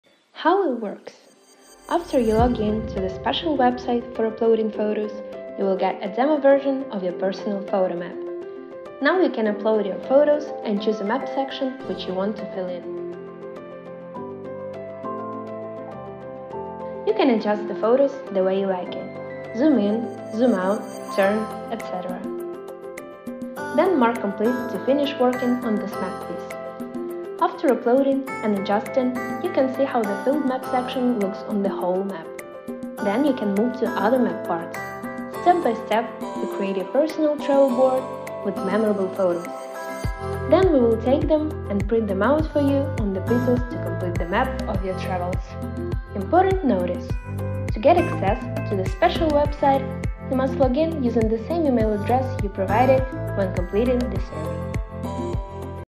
Wooden World Travel Photo Map | ETW | Kickstarter | Tutorial
Absolutely innovative design that is more than just a decorative piece; it's a canvas for your adventures.

After you log in to the special website for uploading photos you will get a demo version of your personal Photo Map.

Now you can upload your photos and choose a Map section which you want to fill in. You can adjust the photos the way you like it (zoom in/out, turn etc.). Then mark “complete” to finish working on this Map piece.

After uploading and adjusting you can see how the filled Map section looks on the whole Map. Then you can move to other Map parts. Step by step you create your personal travel board with memorable photos.

Then we will take them and print them out for you on the pieces to complete the Map of your travels.

- Transform Your Travels into Timeless Art with our Stunning Travel Photo Map Design -

Go to our Kickstarter page and pledge on this project to be among the first with the best offer of the season